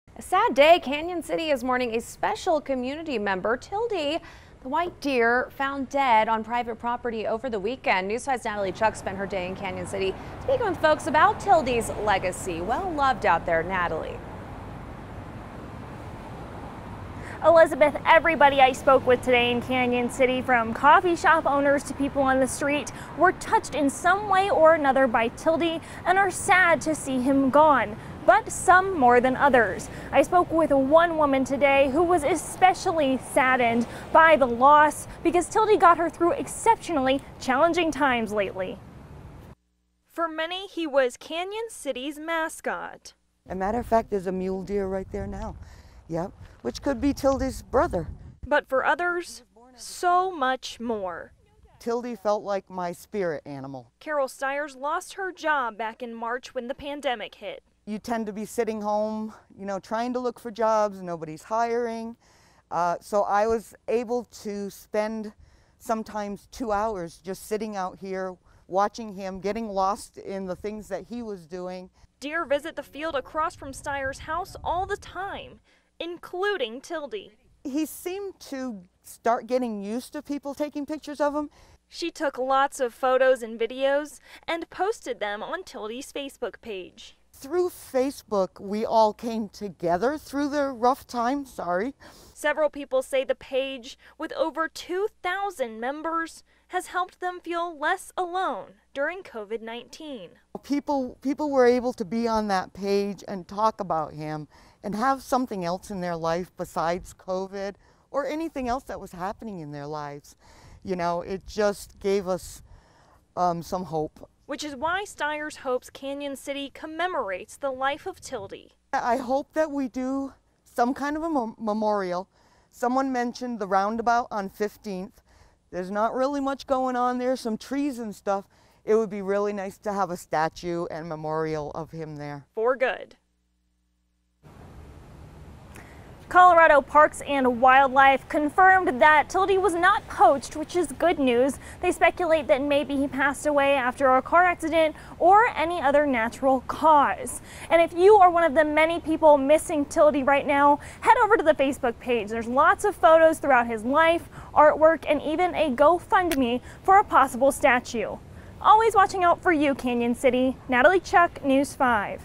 White deer in Canon City dies
TILDE… THE WHITE DEER… FOUND DEAD ON PRIVATE PROPERTY OVER THE WEEKEND